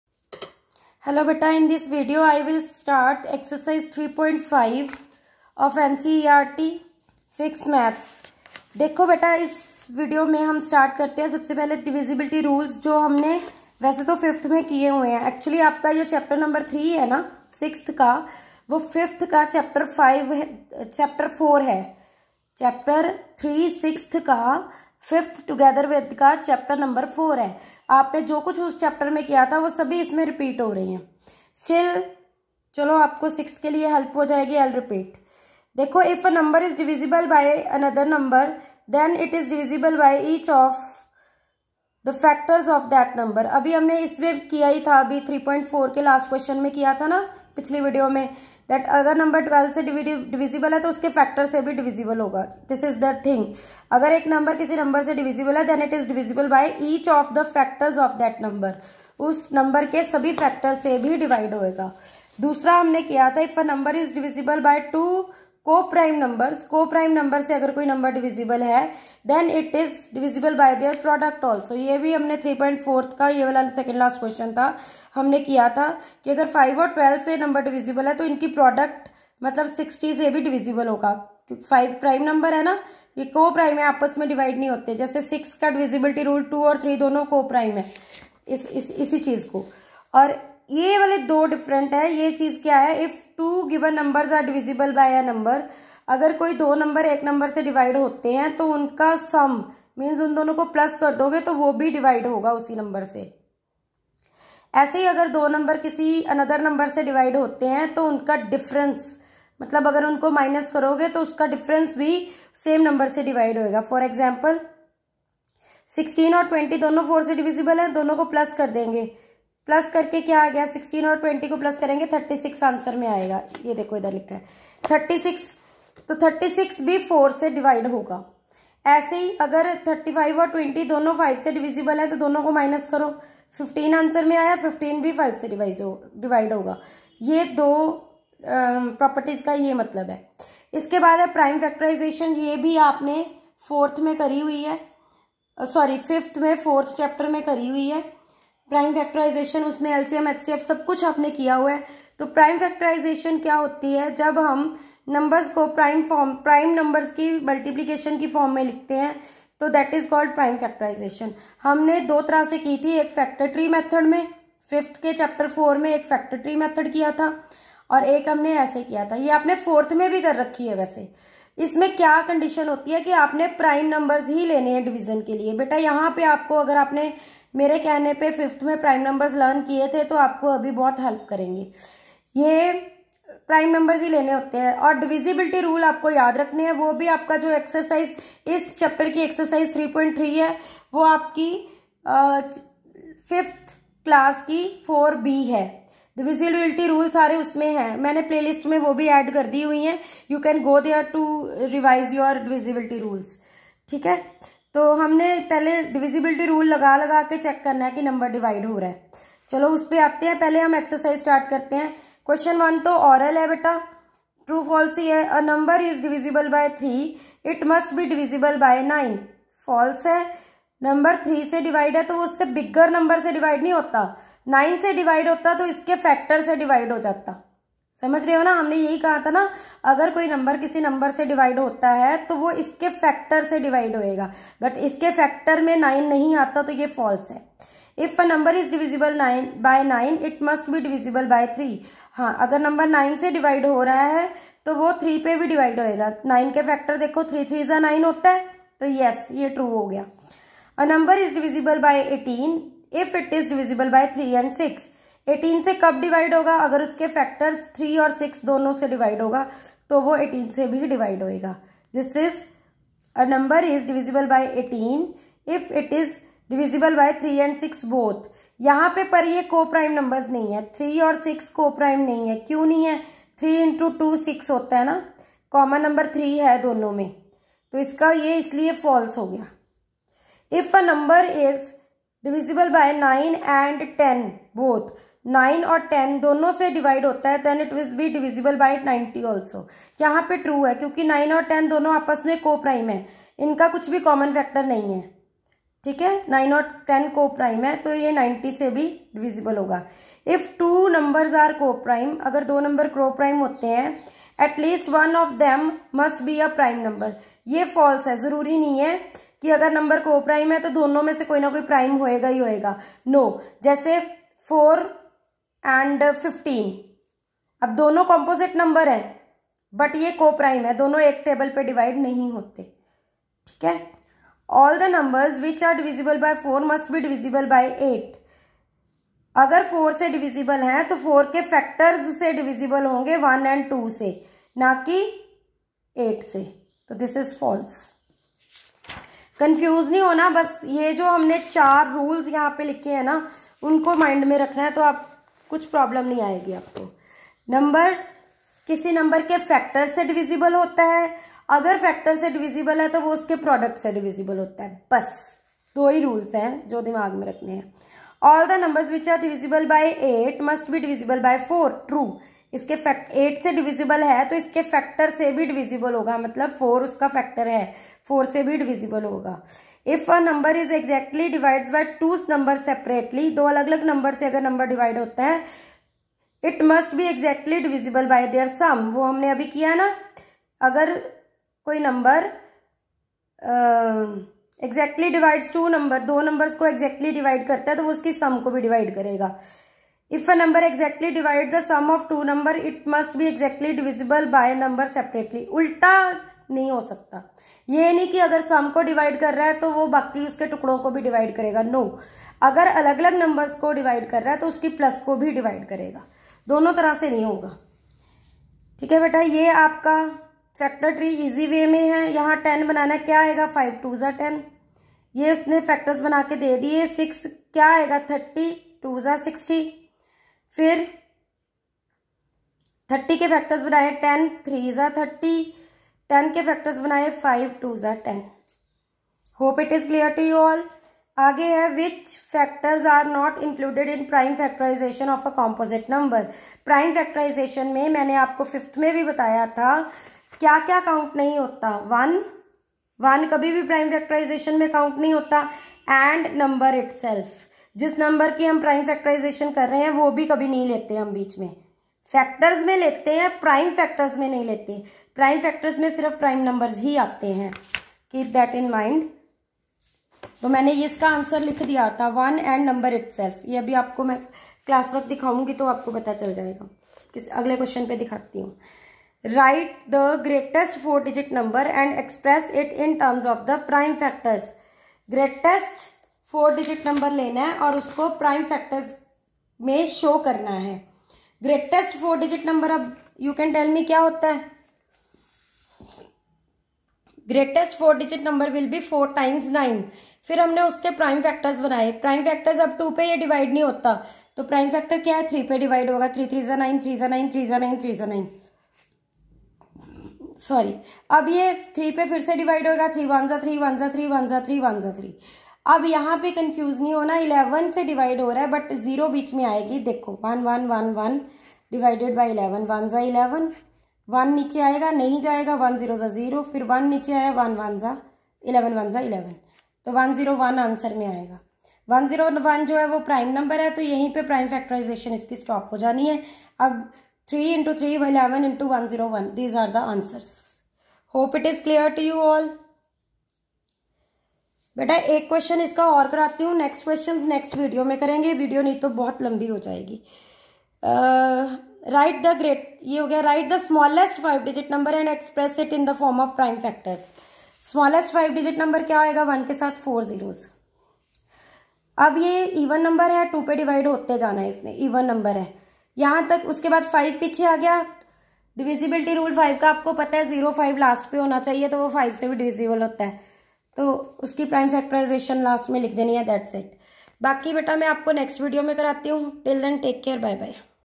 6th maths, NCERT, Ch 3, Ex 3.5, Q1 to Q5 explained
ex 3.5 ,  Q 1 to Q5  explained divisibility rules and prime factorization